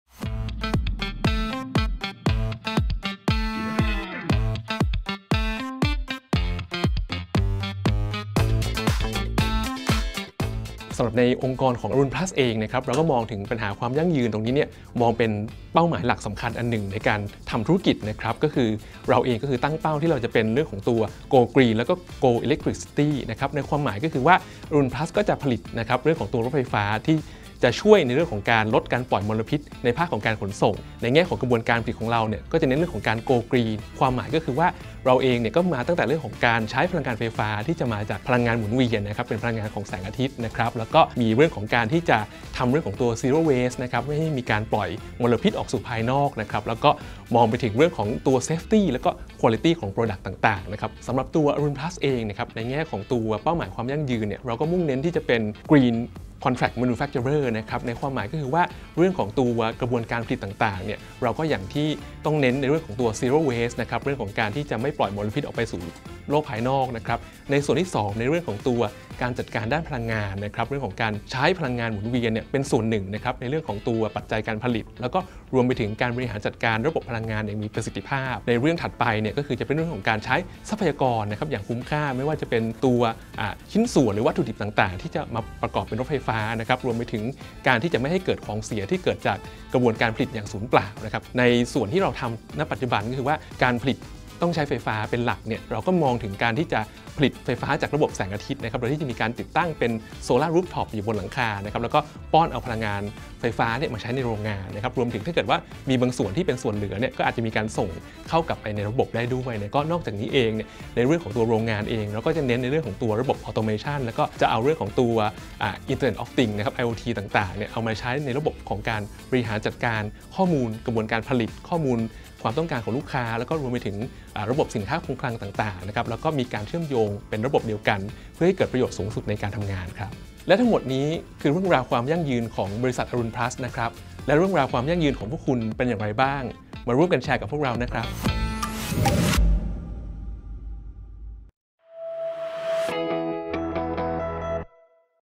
อรุณพลัส ก้าวสู่เป้าหมายความยั่งยืนกับชไนเดอร์ อิเล็คทริค
เป้าหมายหลักสำคัญของ อรุณพลัส คือความยั่งยืนในการทำธุรกิจ เราจึงตั้งเป้าที่จะเป็นองค์กรที่ Go Green และ Go Electricity 🍃

โดยเราจะผลิตรถไฟฟ้าที่จะช่วยในการลดการปล่อยมลพิษ จากการใช้พลังงานไฟฟ้าที่มาจากพลังงานหมุนเวียนพลังงานของแสงอาทิตย์ การจัดการระบบพลังงานอย่างมีประสิทธิภาพ เรื่อง Zero Waste ไม่ให้มีการปล่อยมลพิษออกสู่ภายนอก รวมถึงเรื่อง Safety และ Quality ของสินค้าต่างๆ ด้วยระบบ Automation, Solar Cell, IOT ที่จะเข้ามาช่วยบริหารจัดการให้เกิดความยั่งยืน 💚

📍 แชร์เรื่องราวระบบความยั่งยืนของคุณได้ที่: